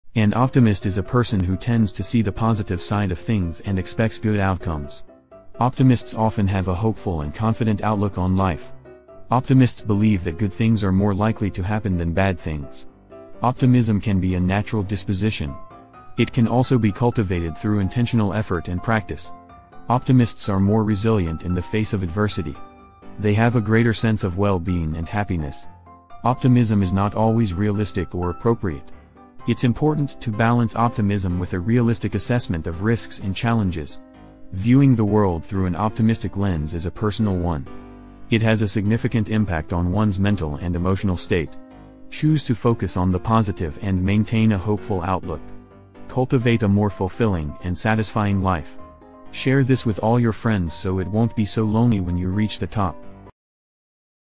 What is an Optimist
An optimist is a person who tends to see the positive side of things and expects good outcomes.

Optimists often have a hopeful and confident outlook on life.

Optimists believe that good things are more likely to happen than bad things.

Optimism can be a natural disposition.

It can also be cultivated through intentional effort and practice.

Optimists are more resilient in the face of adversity.

They have a greater sense of well-being and happiness.

Optimism is not always realistic or appropriate.

It's important to balance optimism with a realistic assessment of risks and challenges.

Viewing the world through an optimistic lens is a personal one.

It has a significant impact on one's mental and emotional state.

Choose to focus on the positive and maintain a hopeful outlook.

Cultivate a more fulfilling and satisfying life.

Share this with all your friends so it won’t be so lonely when you reach the top!